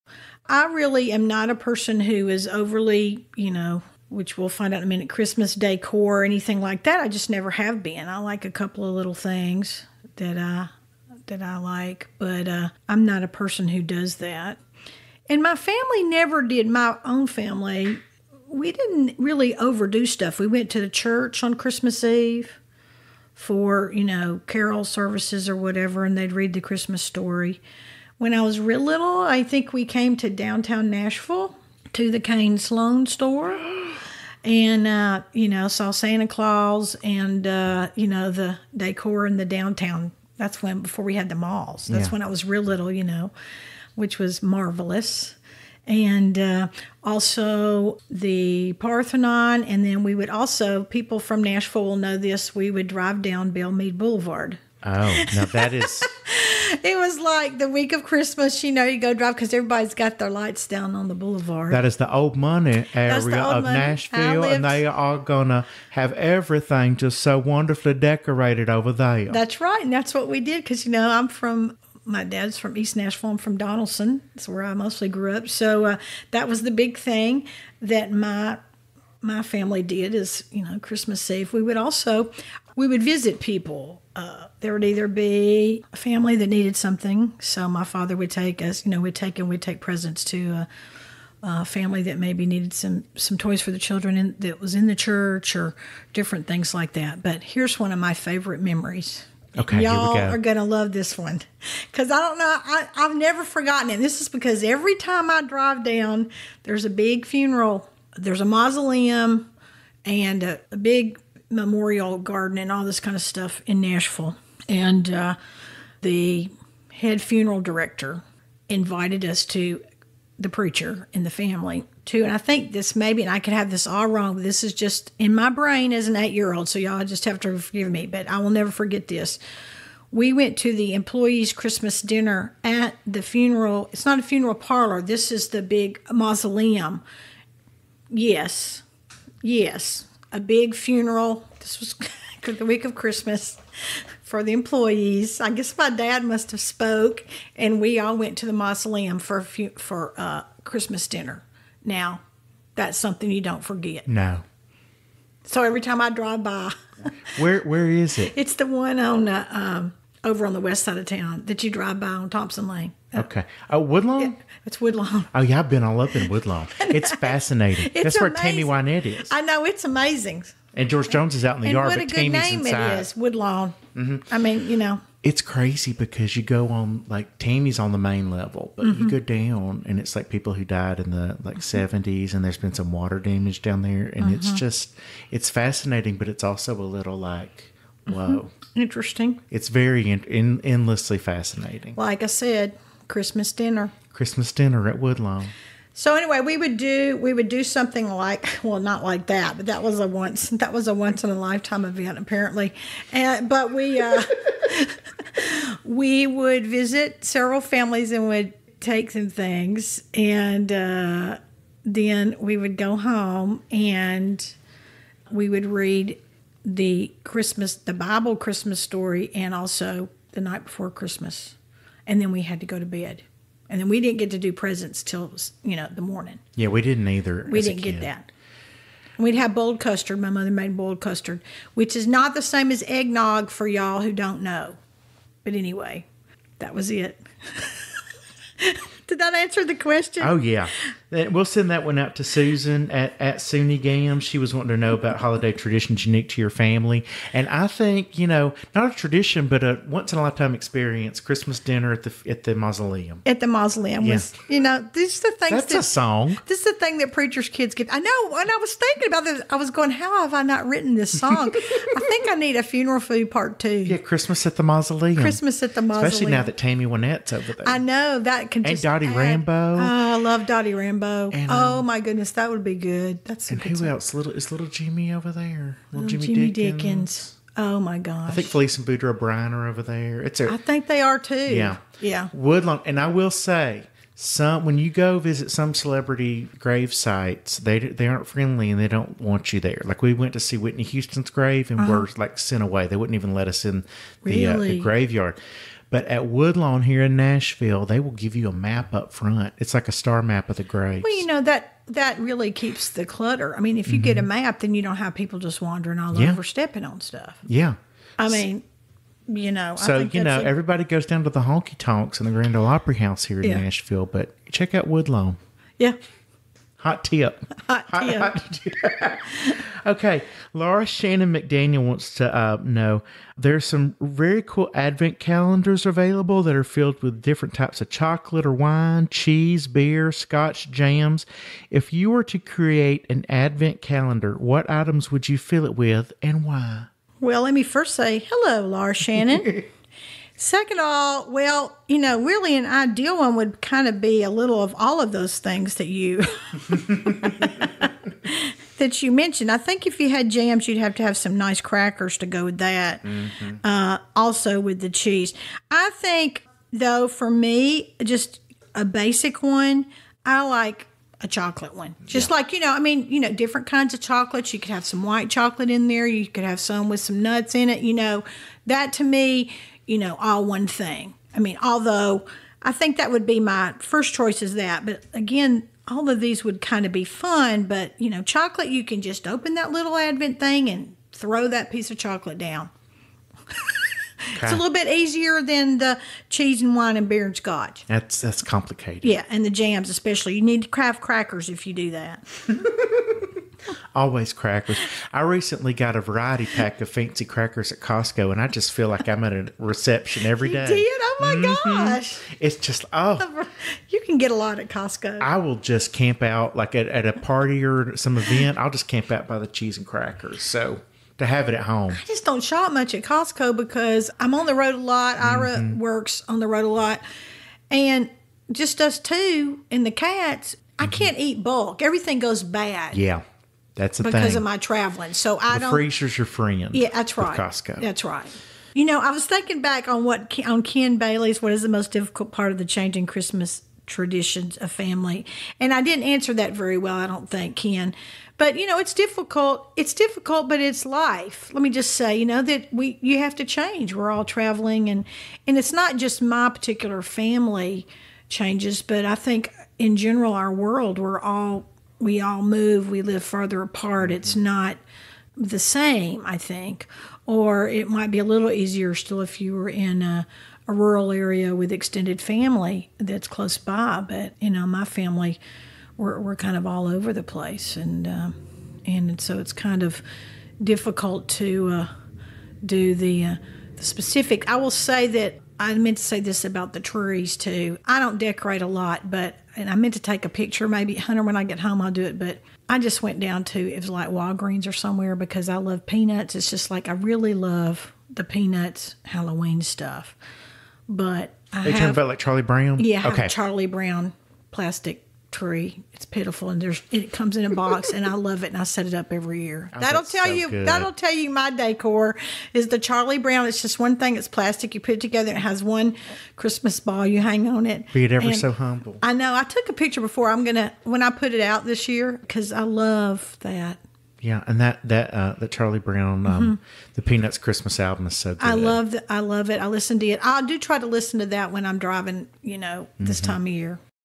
Kate Campbell Six Questions December 2019 Part Two
Kate talks about her family’s Christmas traditions then and now and also explains how the cartoon Peanuts fits into her Christmas decorating! Part three, coming soon!